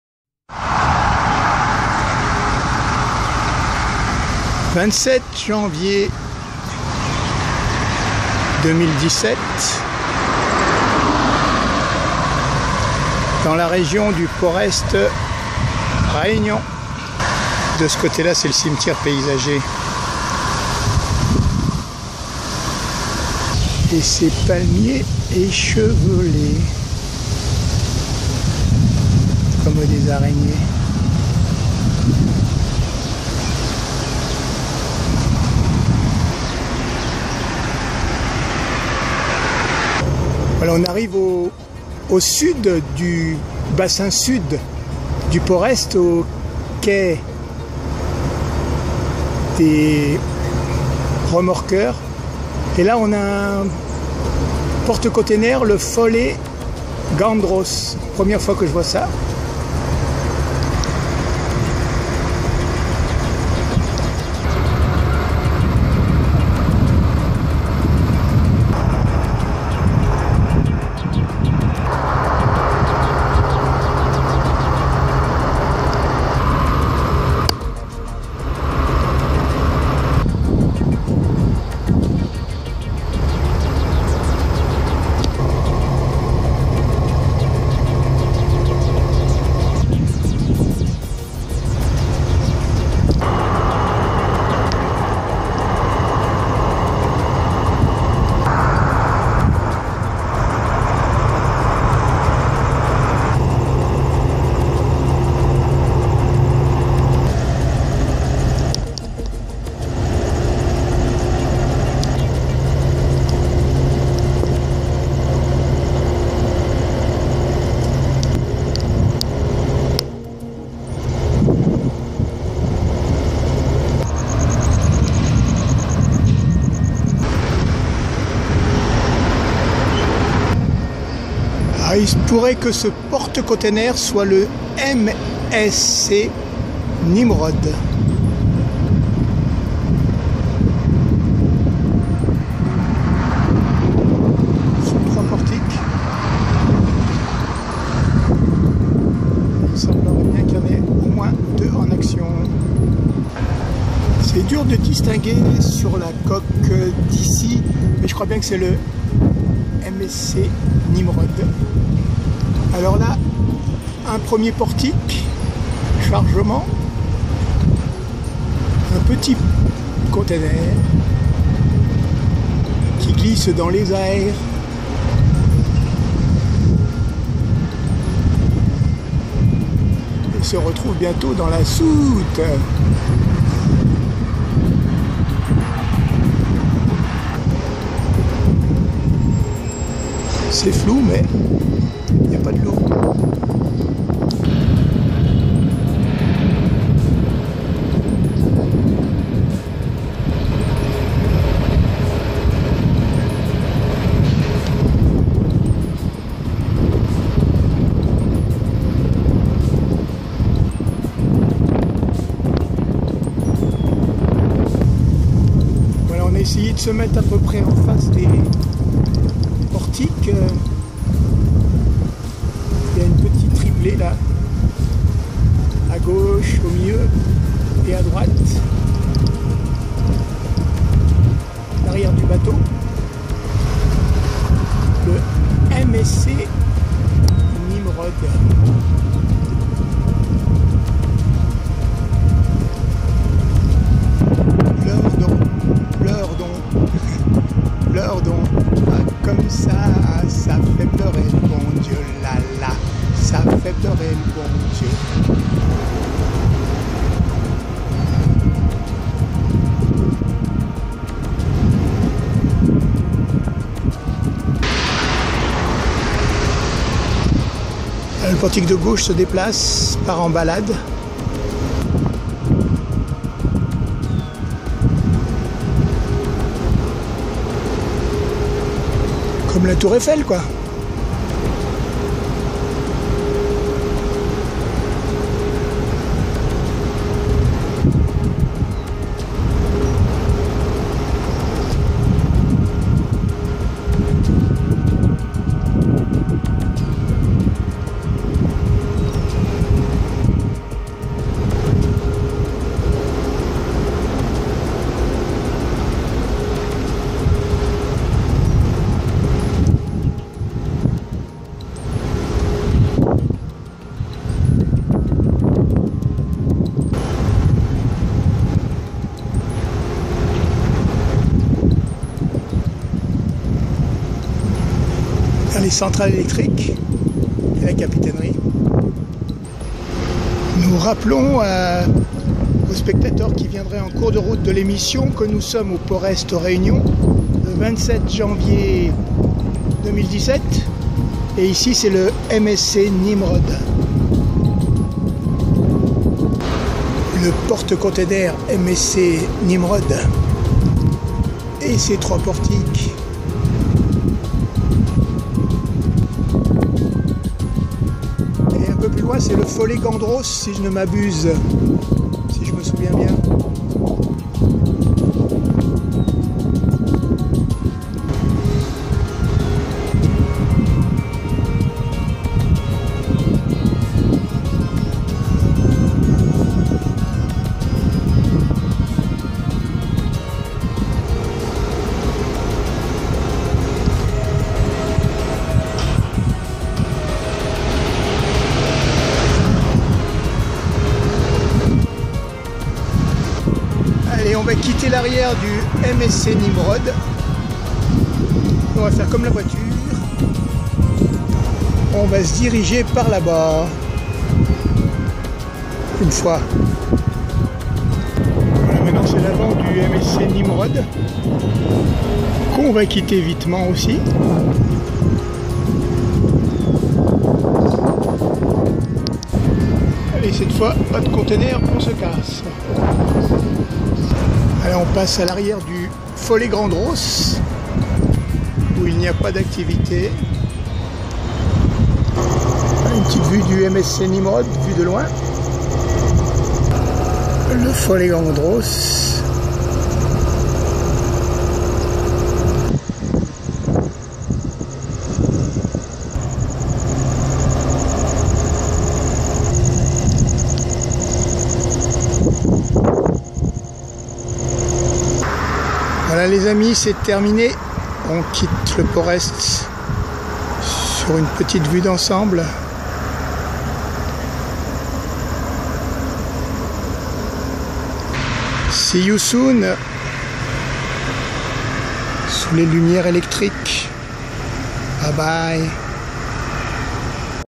PORT EST RÉUNION,Folegandros et Msc.Ninogo,27 JANVIER 2017.
Deux Portes Conteneurs.